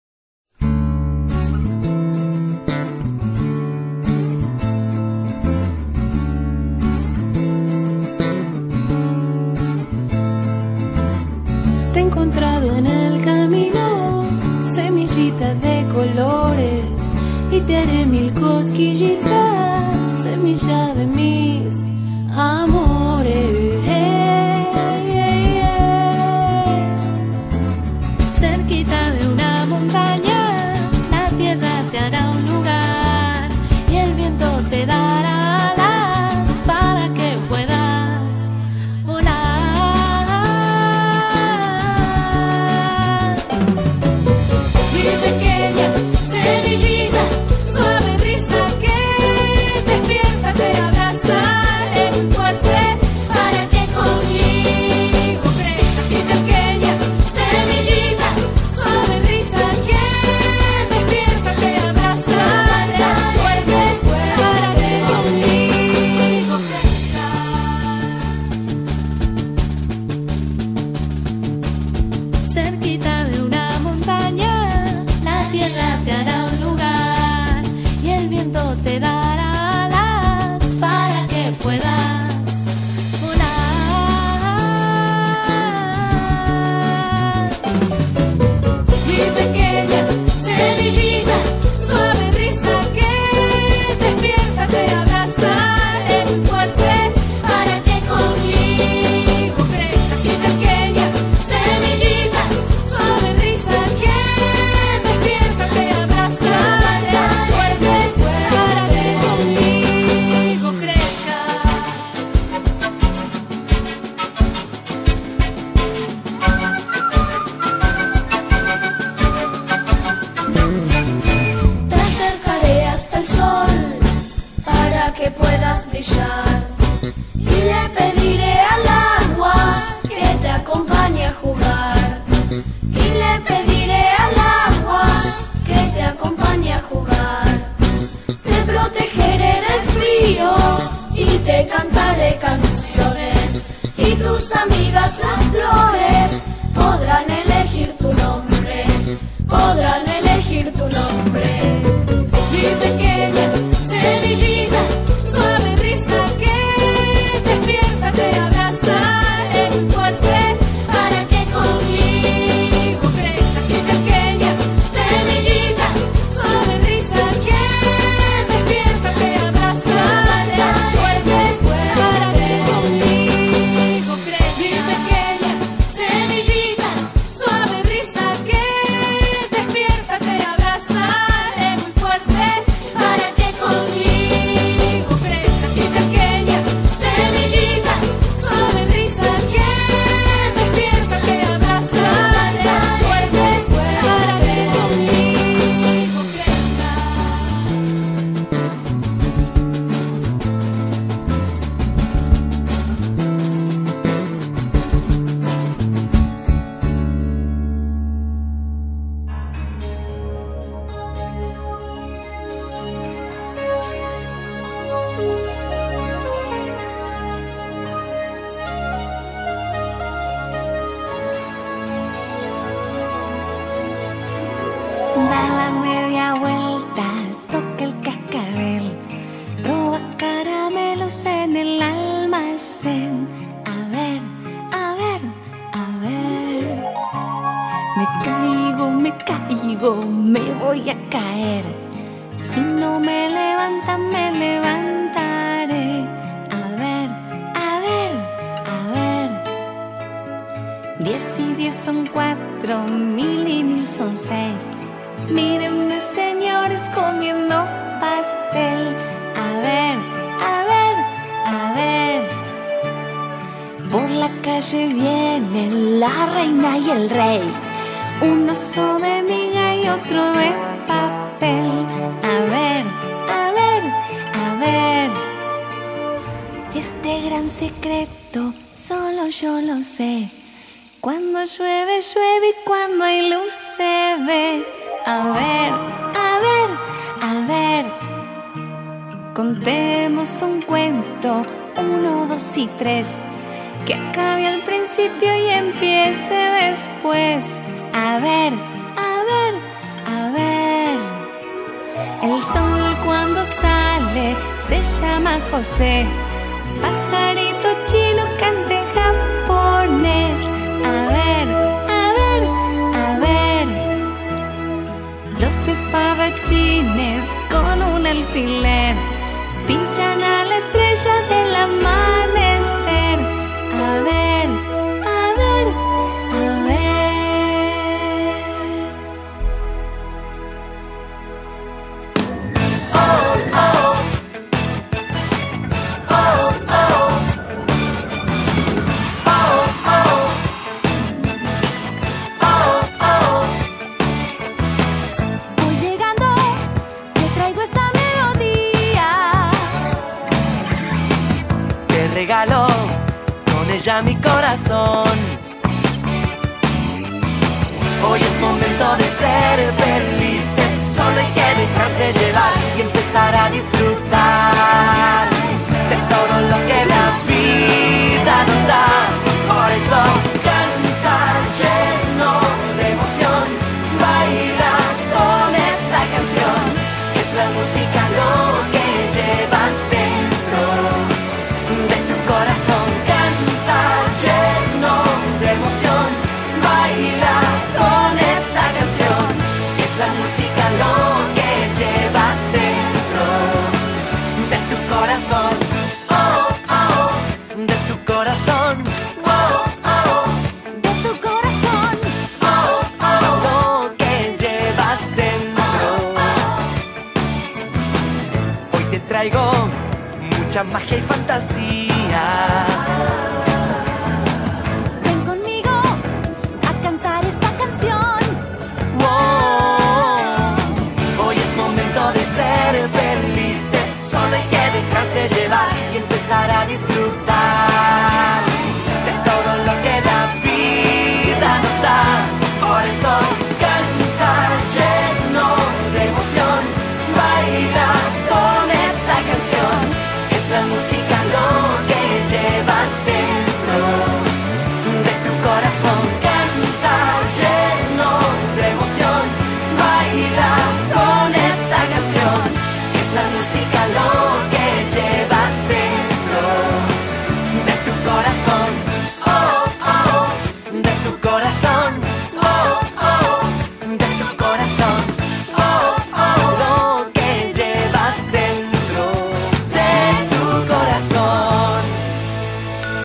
Período de Inicio 2023 -Sala de 2 t.m.- JARDÍN UNNE
Viernes 28 de abril de 2023

Realización de la 2da. Reunión y Taller de padres en salita de 2 años turno mañana, para hablar sobre el proceso de aprendizaje de los niños en el período de inicio del ciclo lectivo y las expectativas de logro  que guiarán el 2023. Entrega del 1er informe evaluativo, acompañado con el presente video que sostiene con imágenes dicho informe escrito.
¡Gracias familias por estar siempre!
¡Gracias familias por acompañar!